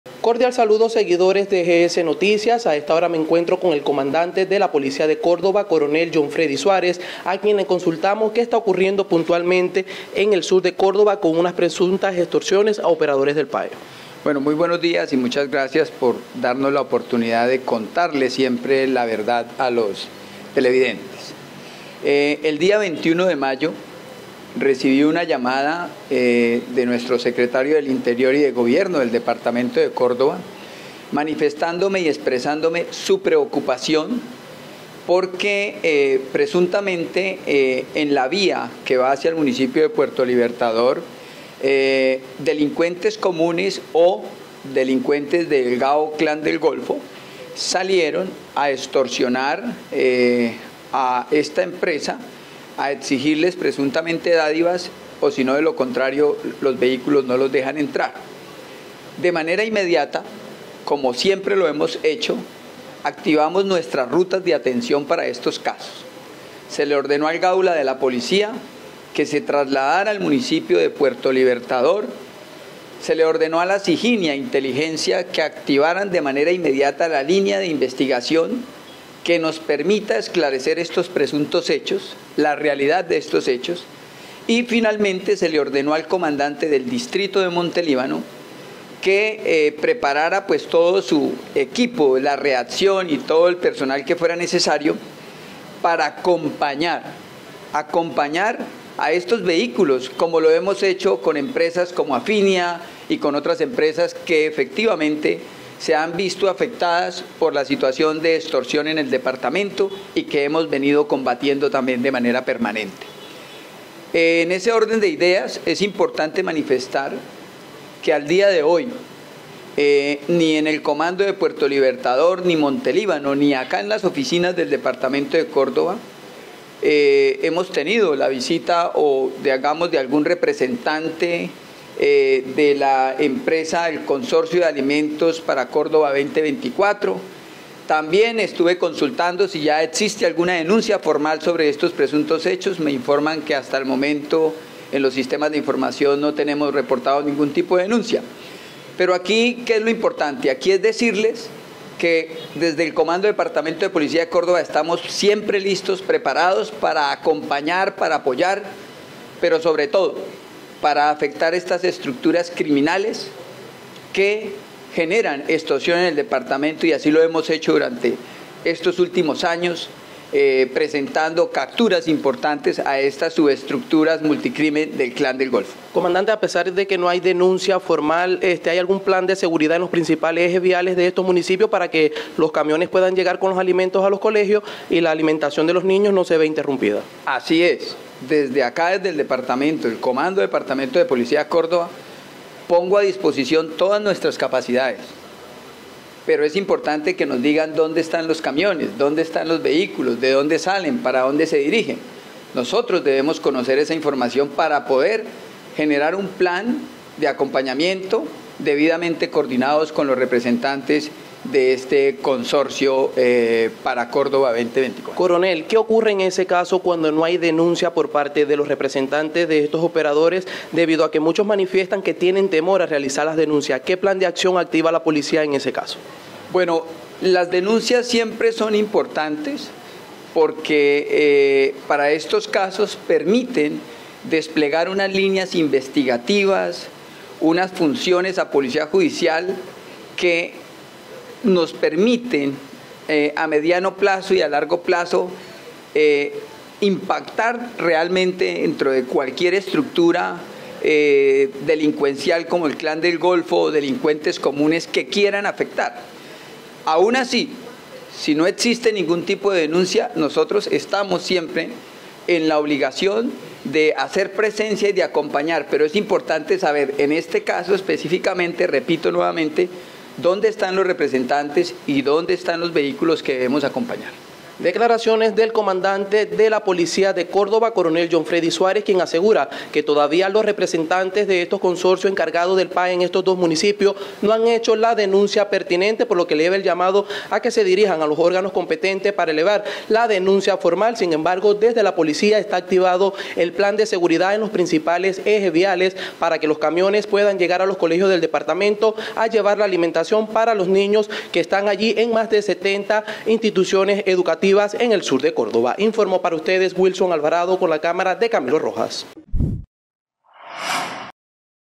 Policía investiga presunta amenaza extorsiva a operador del PAE
Córdoba | El @GaulaPolicia y detectives de la Sijín, asumen investigación por presunta amenaza extorsiva a operador del PAE en Córdoba y que dejó sin comida a niños de 70 instituciones educativas de esta regional.